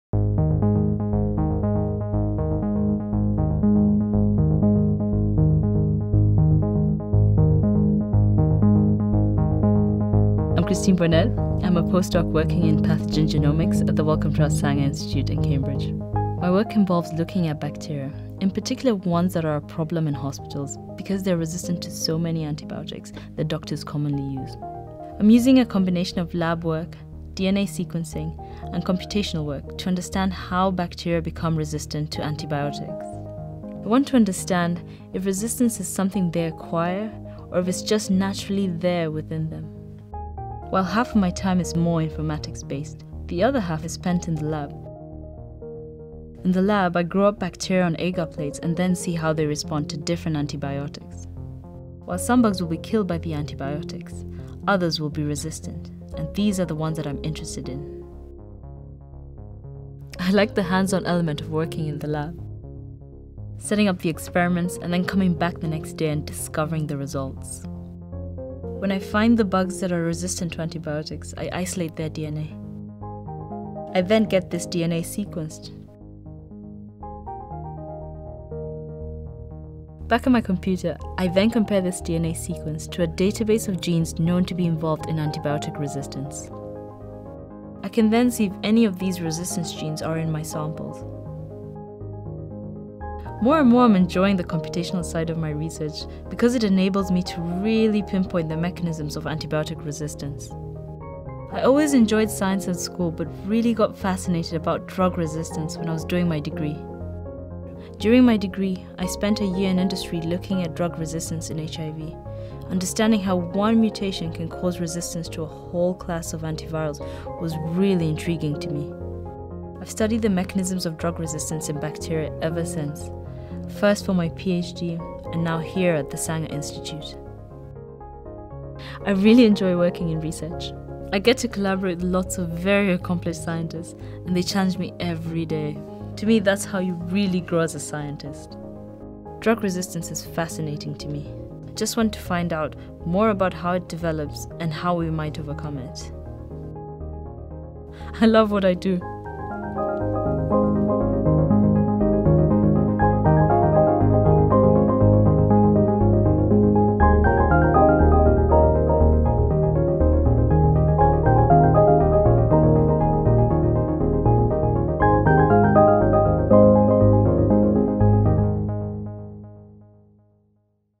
My career in genomics: antibiotic resistance
In this film Christine Boinett talks about her research at the Wellcome Trust Sanger Institute looking at antibiotic resistance in bacteria. This is one of a series of films providing a unique insight into different careers in the field of genomics.